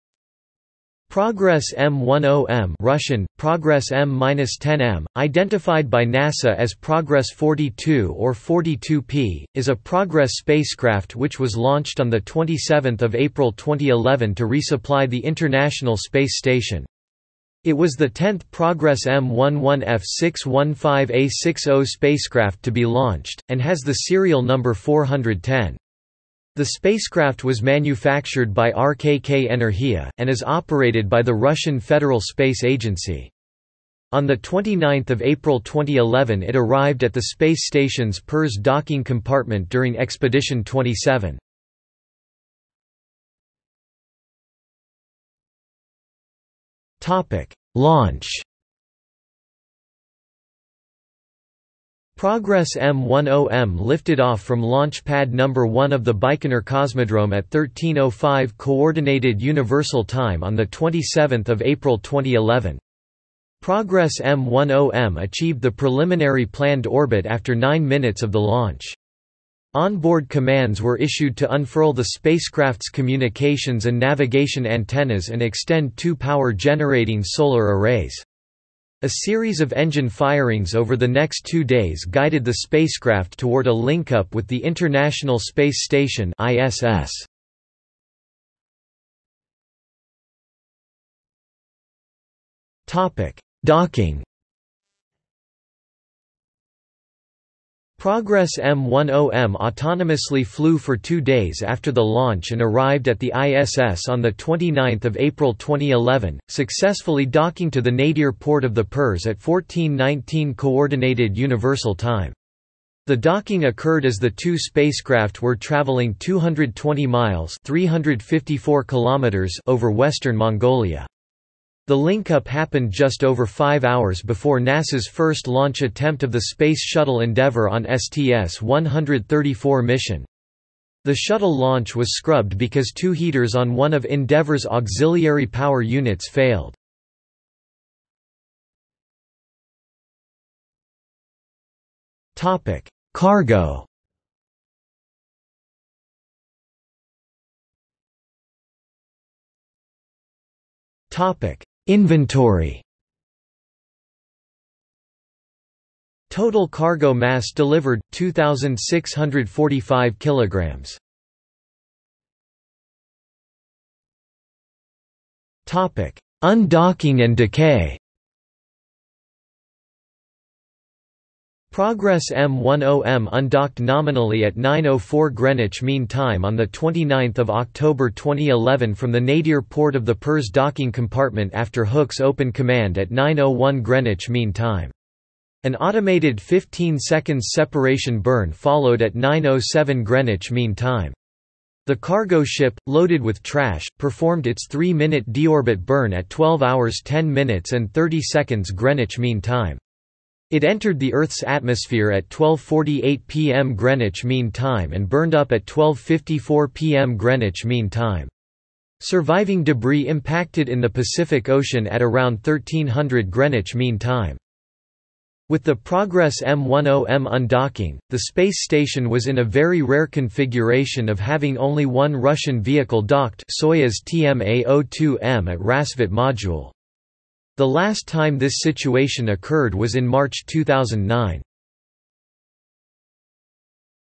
Progress M-10M | Wikipedia audio article
00:00:43 1 Launch
00:01:29 2 Docking
00:02:18 3 Cargo
00:02:27 3.1 Inventory
00:02:42 4 Undocking and decay

- Socrates

SUMMARY
Progress M-10M (Russian: Прогресс М-10М), identified by NASA as Progress 42 or 42P, is a Progress spacecraft which was launched on 27 April 2011 to resupply the International Space Station. It was the tenth Progress-M 11F615A60 spacecraft to be launched, and has the serial number 410. The spacecraft was manufactured by RKK Energia, and is operated by the Russian Federal Space Agency. On 29 April 2011 it arrived at the space station's Pirs Docking Compartment during Expedition 27.